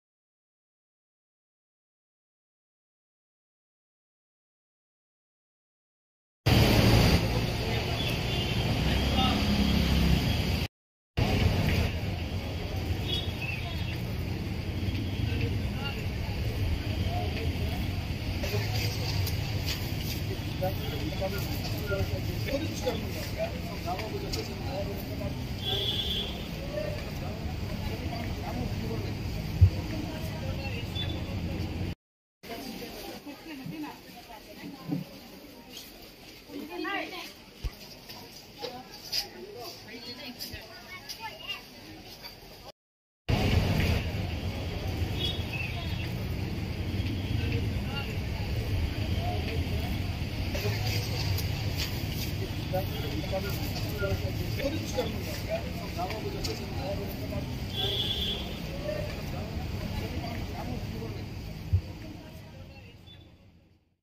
Nagaland Unlock-4, Dimapur glimpse
Nagaland has unlock the pandemic lockdown for 4th times.